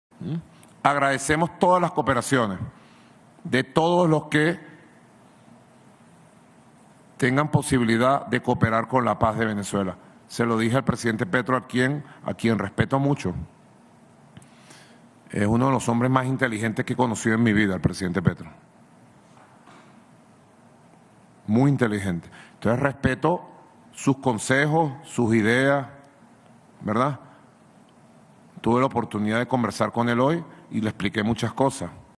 El elogio de Nicolás Maduro al presidente Petro
El presidente venezolano expresó su admiración por Gustavo Petro y confirmó una constante comunicación con él.